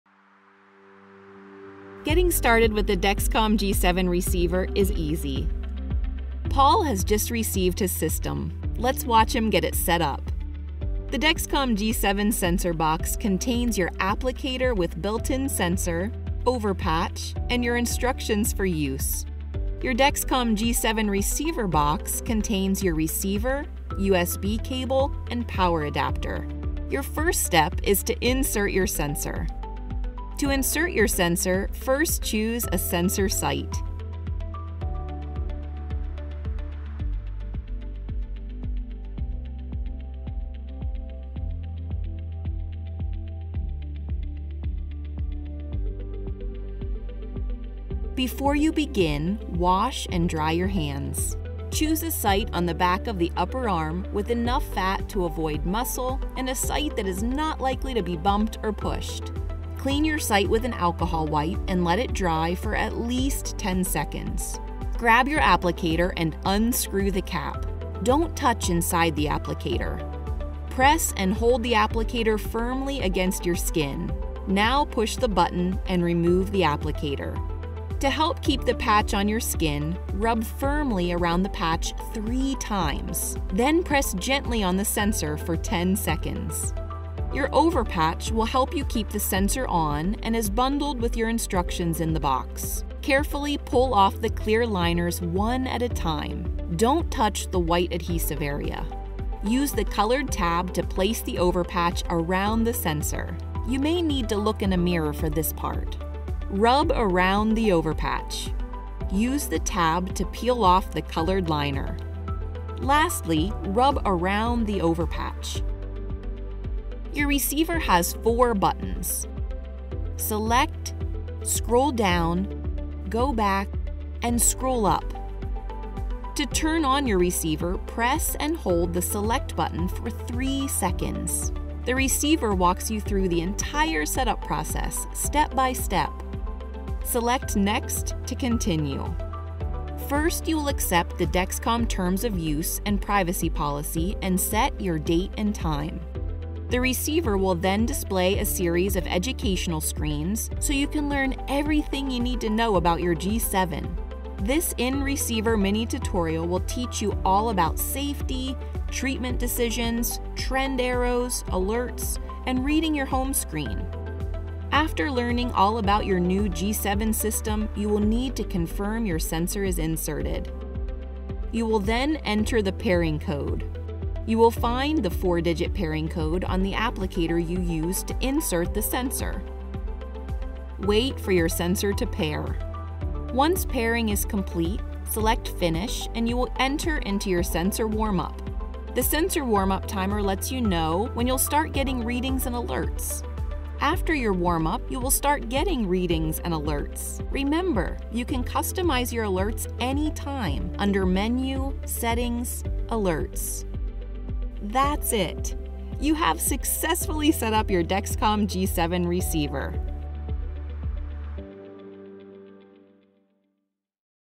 Dexcom G7 Receiver - How to Get Started and Set Up the Receiver
Just getting started with the Dexcom G7 receiver? This video show you how to set up your receiver and insert your sensor.

LBL-021524 Rev001